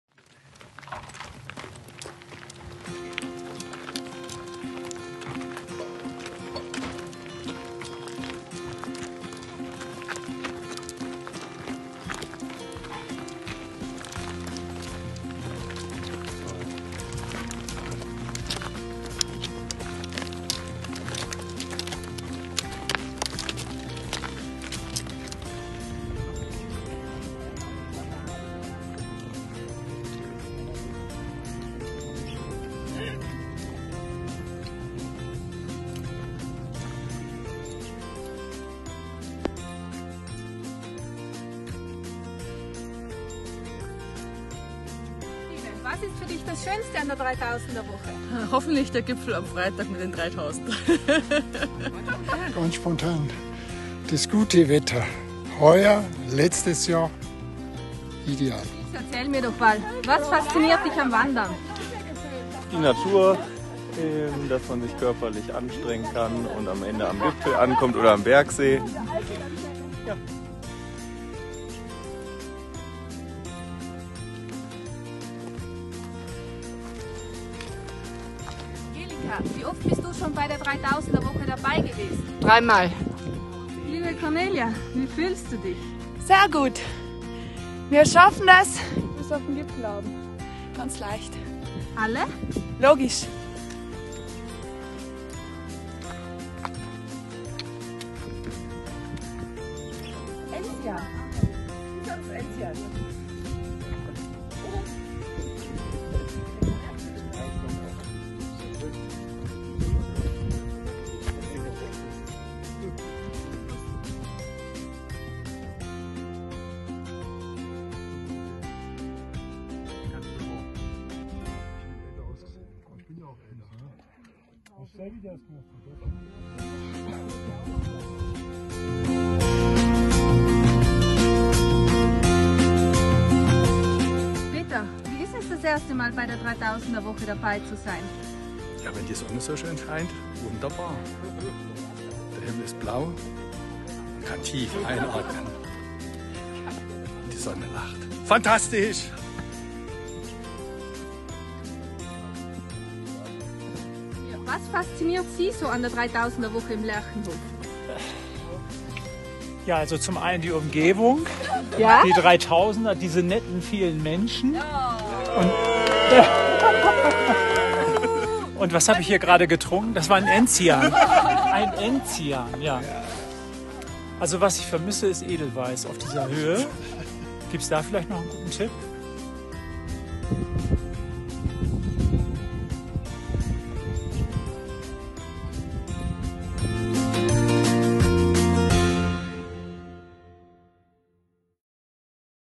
Auf den Dürrenstein (2.839 m)
Was für ein Tag! Am 15. September 2020 machten wir uns auf zur Plätzwiese in den Pragser Dolomiten und eroberten dann den imposanten Gipfel des Dürrensteins! Ein toller Auftakt zur 3000er Woche 2020!